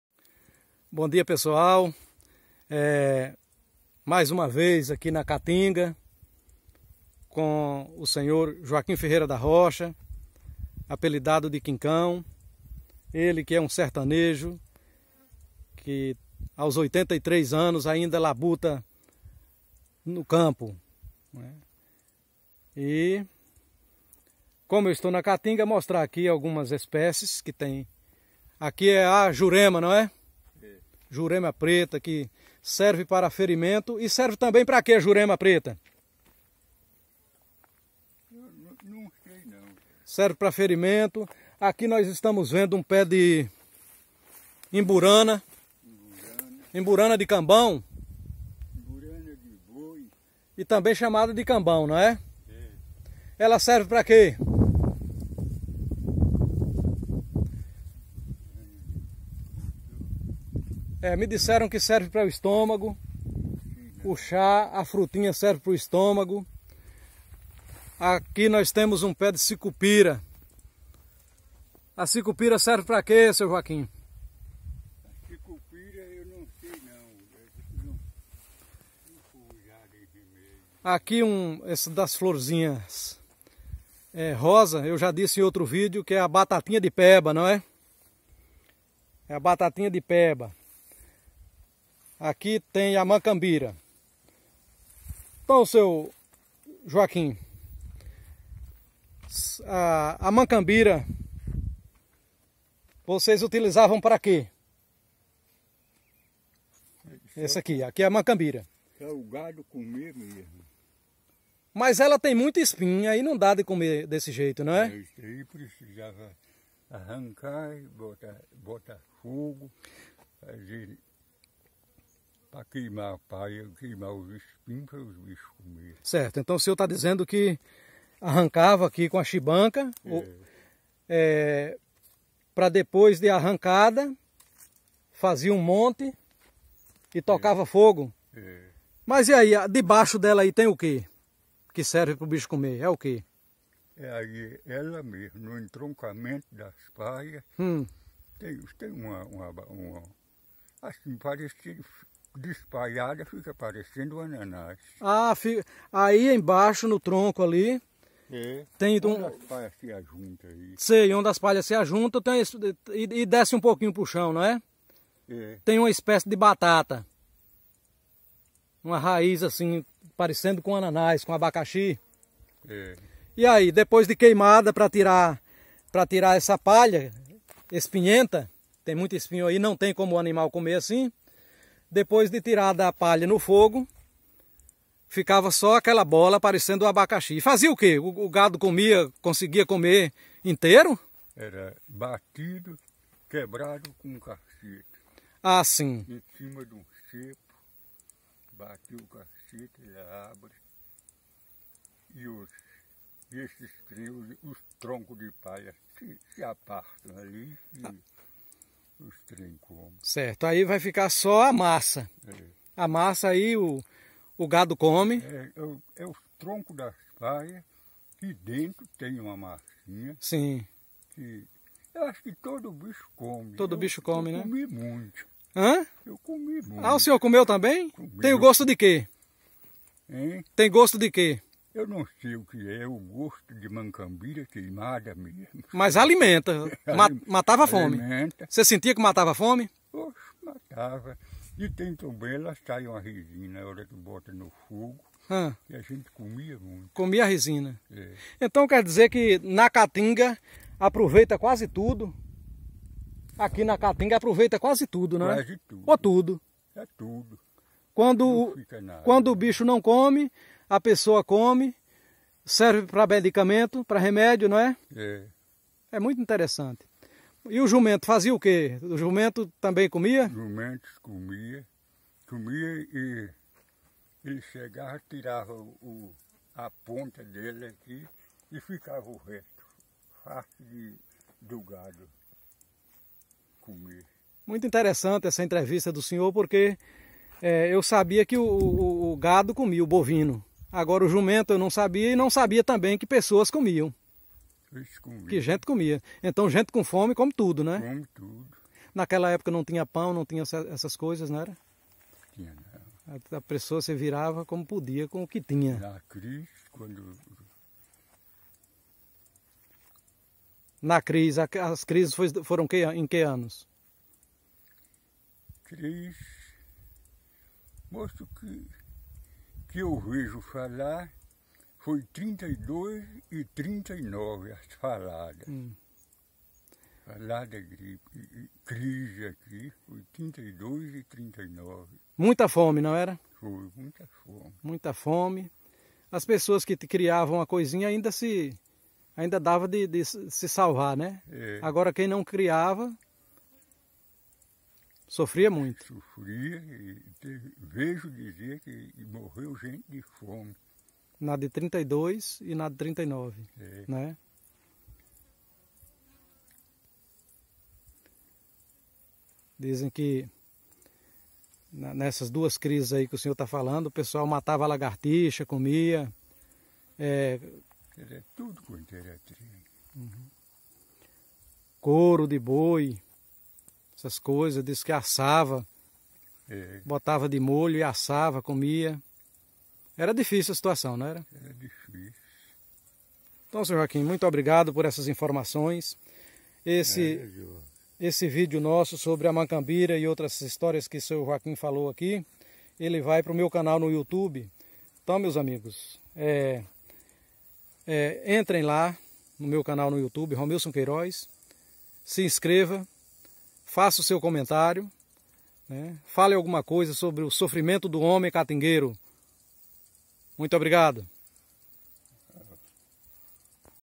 Joaquim Ferreira da Rocha ( Kincão ) e sua história!
Um catingueiro de 83 anos, que abre o coração e faz belos relatos sobre como se virava para sobreviver na caatinga. As dificuldades eram imensas para os moradores deste bioma, mas para Ele era muito mais difícil, pois tem 14 filhos para dar o sustento!